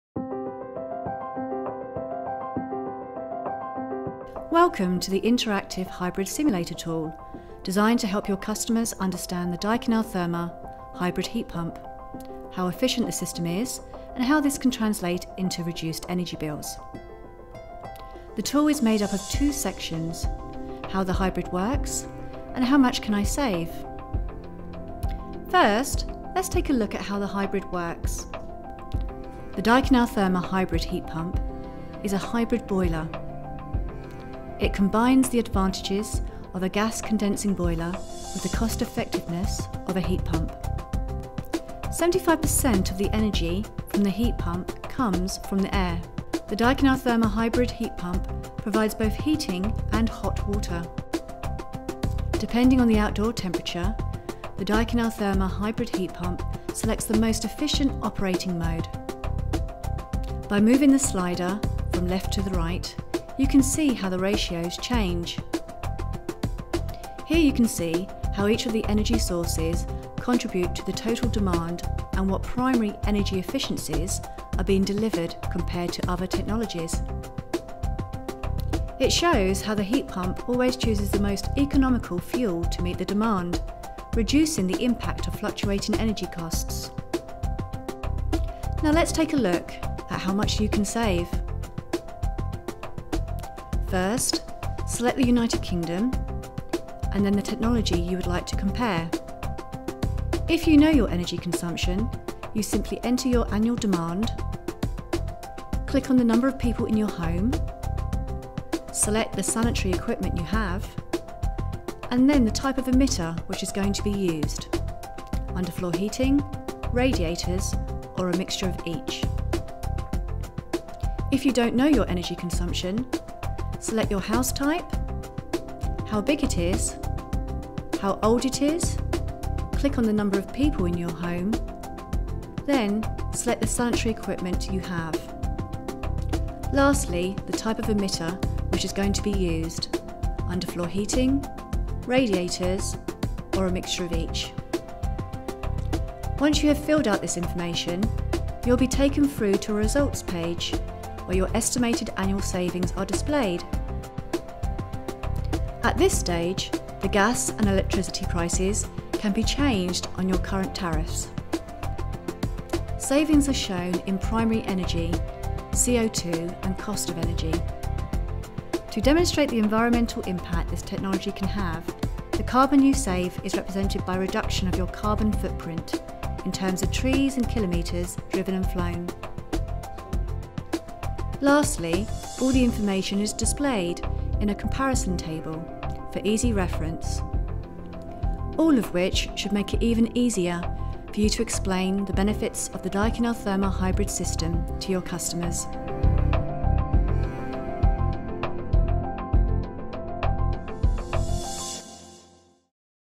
Hybrid heat pump simulator demo Animations English
Compare the Daikin Altherma hybrid heat pump with another system and see how much you can save on your energy bill.